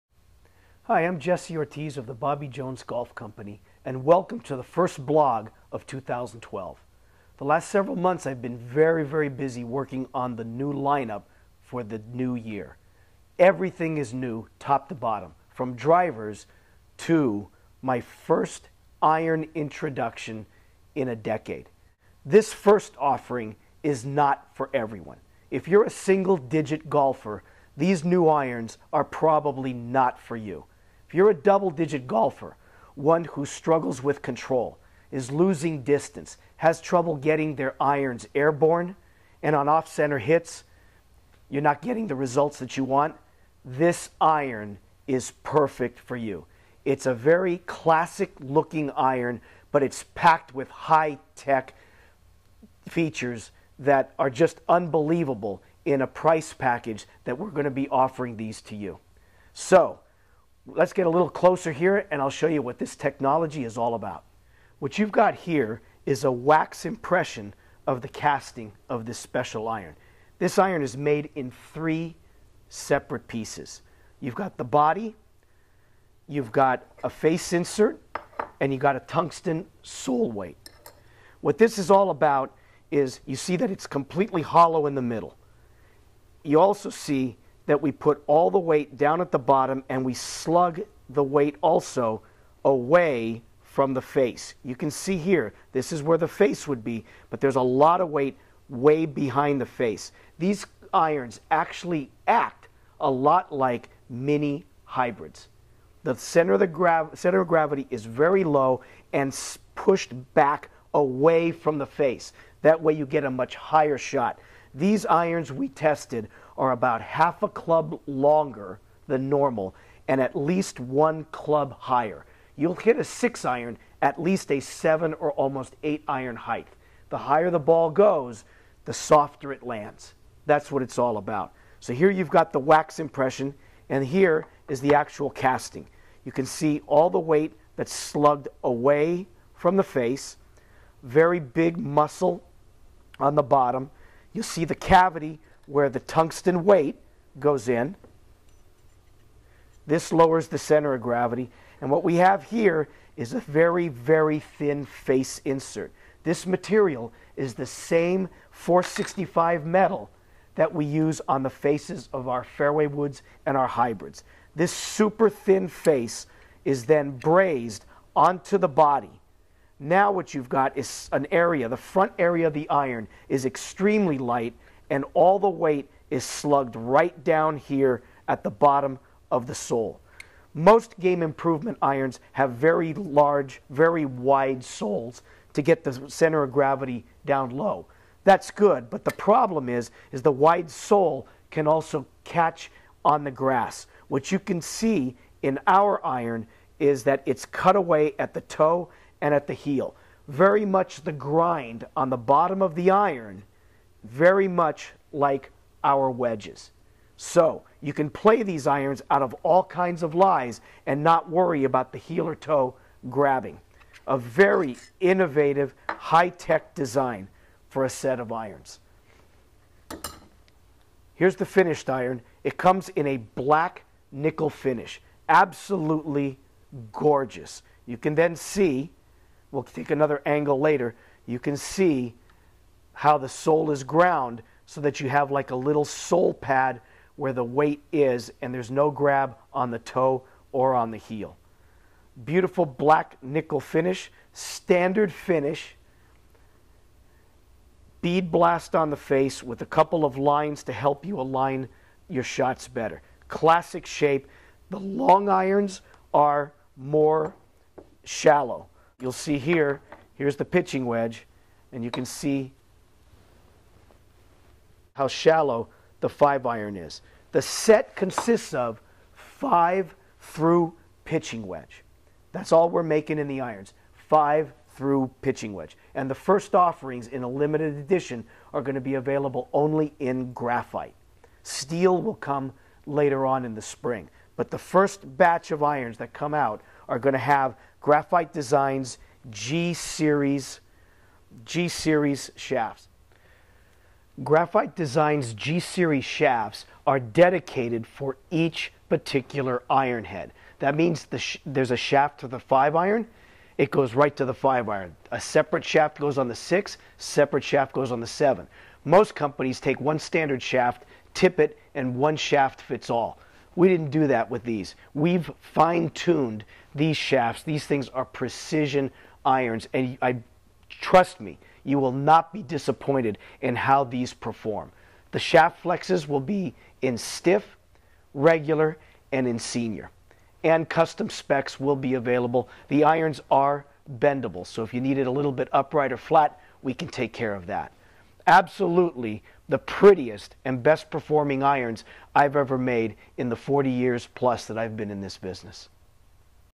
Bobby Jones Golf: Iron Set - Introduction
Jesse Ortiz of Bobby Jones Golf proudly introduces his new irons, available now for purchase as a set of six.